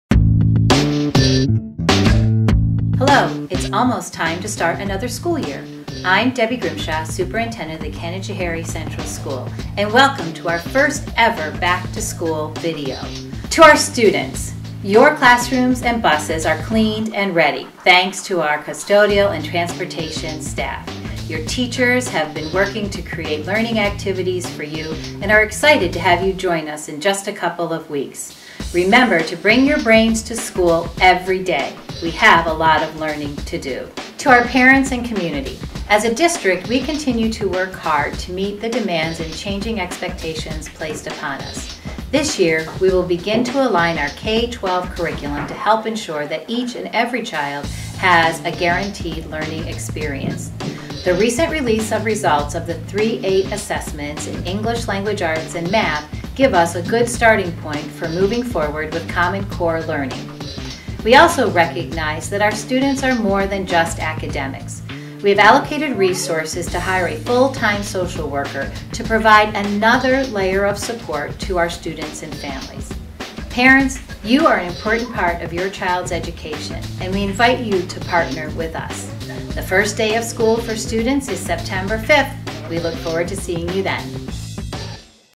Canajoharie: Welcome to the 2013-14 School Year
A message from the Canajoharie Superintendent, Debbie Grimshaw.